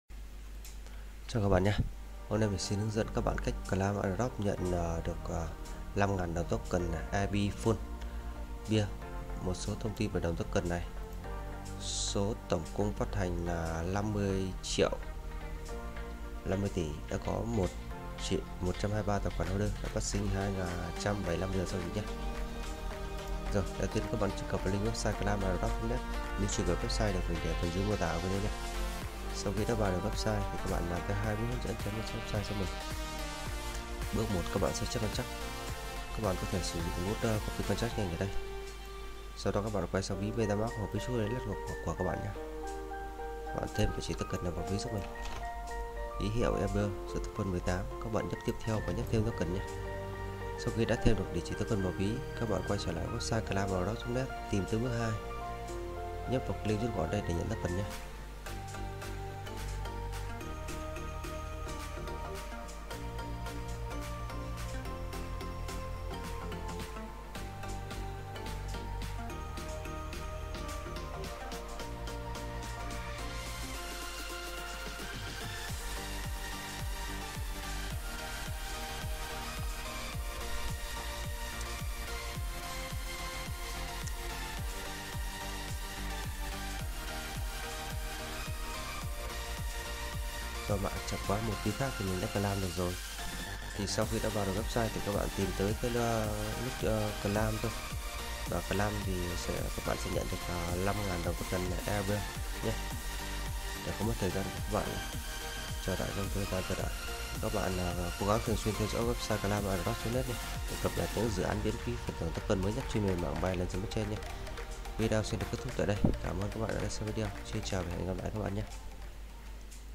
Claim Airdrop Nhận 5,000 Token e-beer.fun e-beer(ml), Mạng BSC Ví Trustwallet, MetaMask
+ Bước 1: Sao chép Contract :  0x09776ef729b0c0d07196f8b2deaa93cc83cef17a

+ Bước 2: Truy Cập Web Tại Đây Click ( Claim Airdrop ) Nhận Token